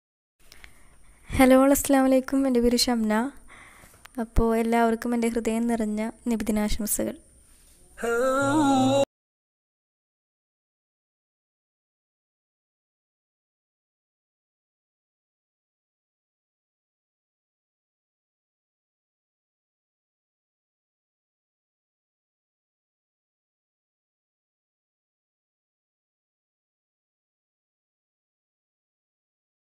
എല്ലാർക്കും നബിദിനാശംസകൾ | Muhammed (saw) in 3 different styles in thuluth
Hello all, Assalamu alaikum
I am shamna, a self taught calligraphy artist. I like to experiment in islamic traditional art forms..   ❣️❤️